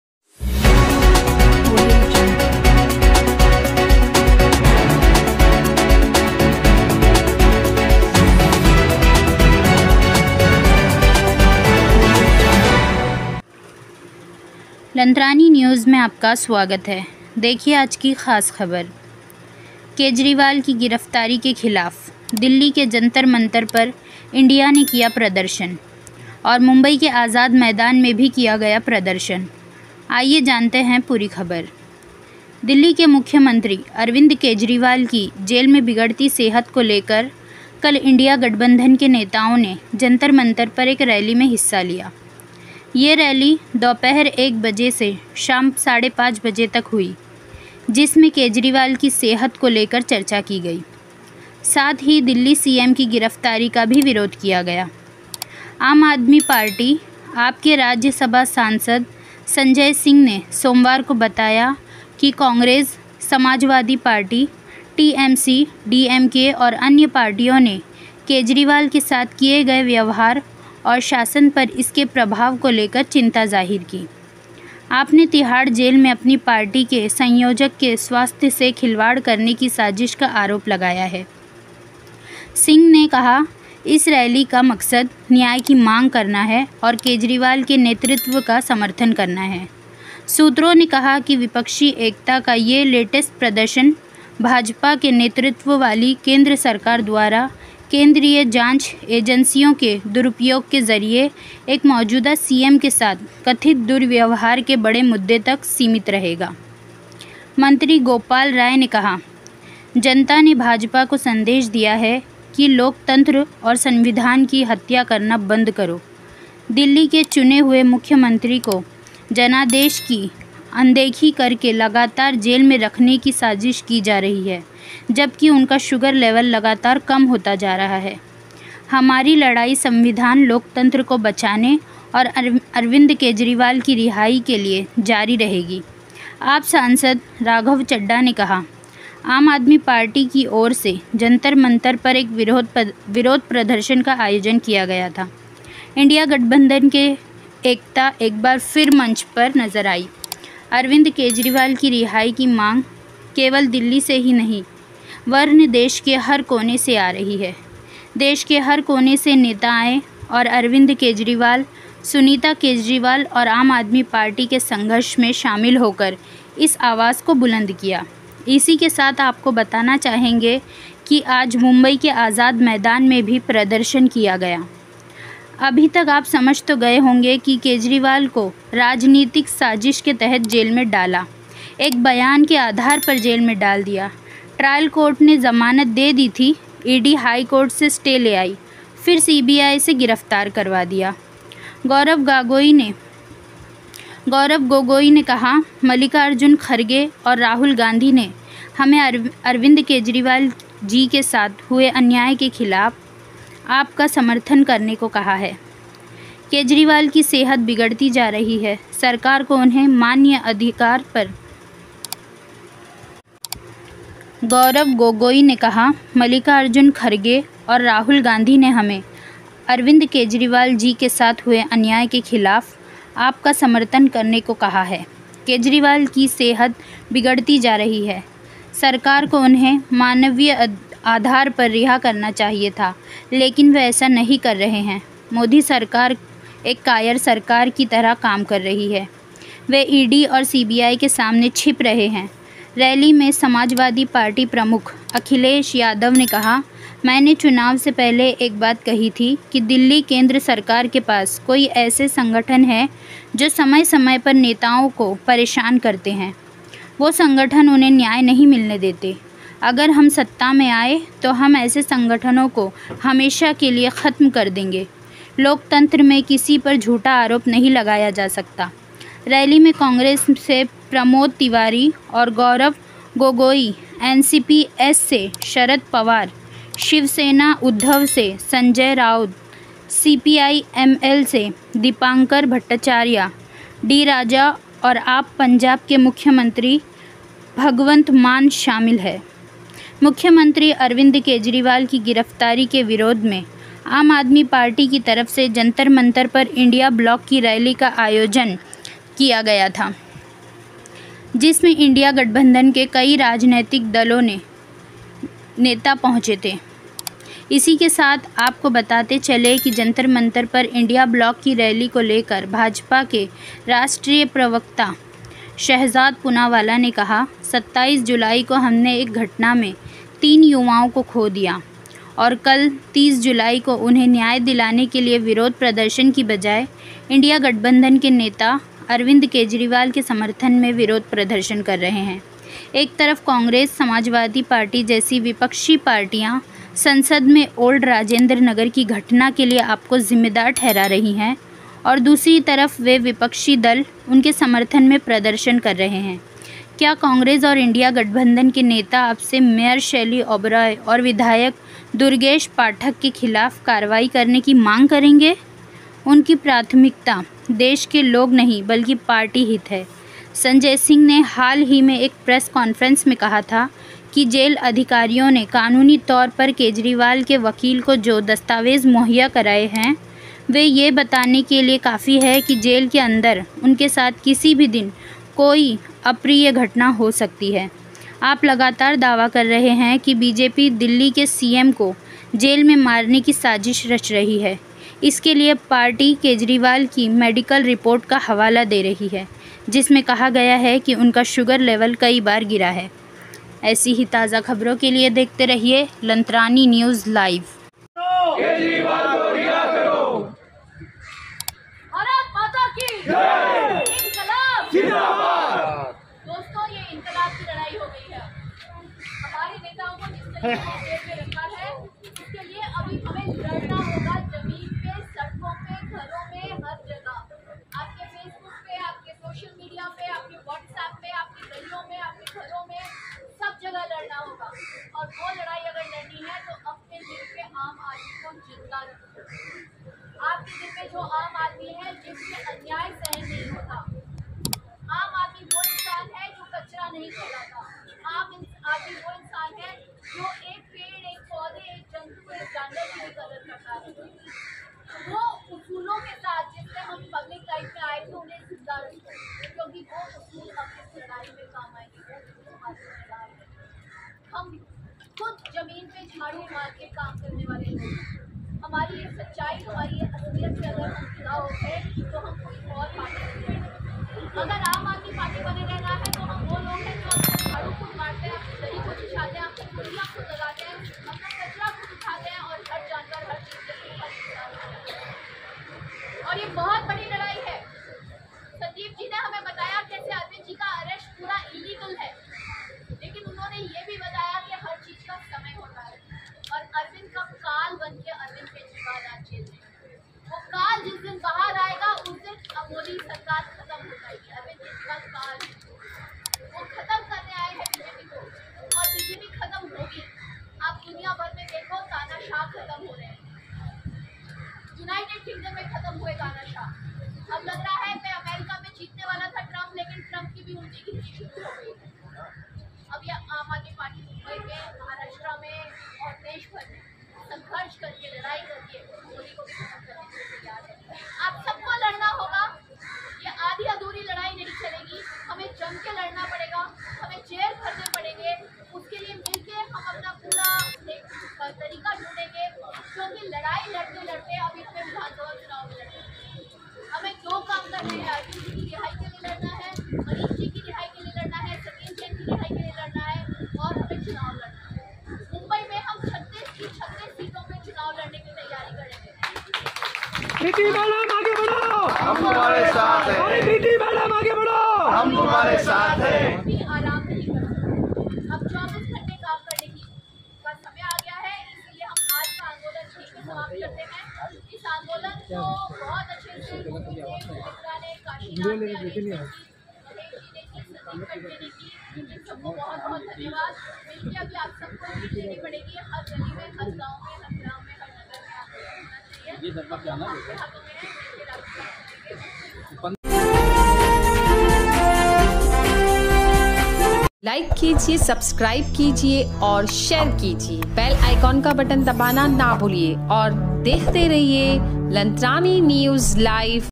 Arvind Kejriwal Ki RIHAI Ko Lekar EkJUT Hua India Gatbsndhan. Janter Manter Per DHARNA Patdarshan
For contacting us :

Phone number :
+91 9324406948 (Mushir ansari)- LANTRANI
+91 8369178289(Milind Khar Patil)- maha Mumbai